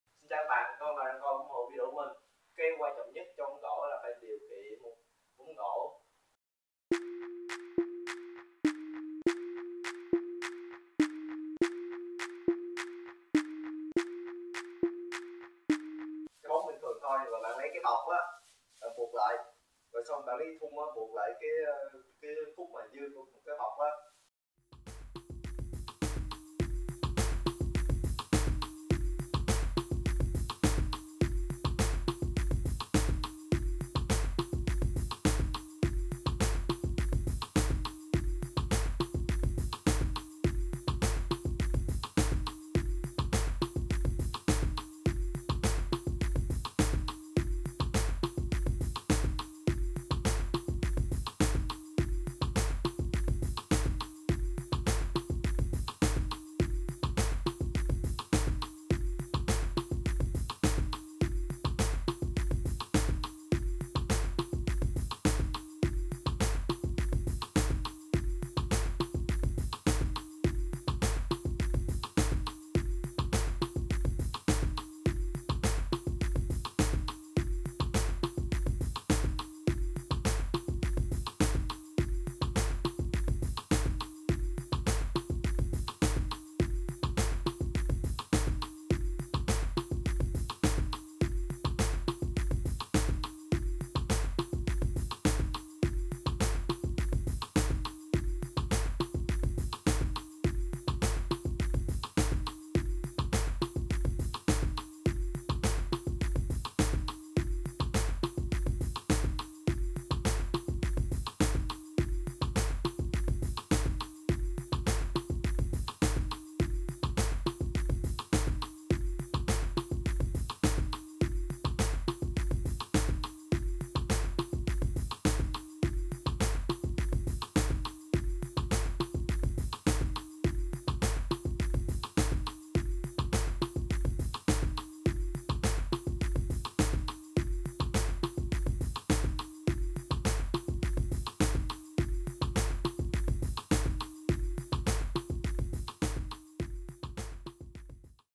[ HIEU VIZZLE ] MẸO ĐỂ ĐIỀU KHIỂN BÓNG GIỎI !!!
BÀI TẬP NÀY BẠN PHẢI TẬP CÁNG NHANH CÀNG TỐT. BẠN PHẢI BỎ THỜI GIAN RA TẬP LUYỆN ĐỂ MỚI THẤY TIẾN BỘ.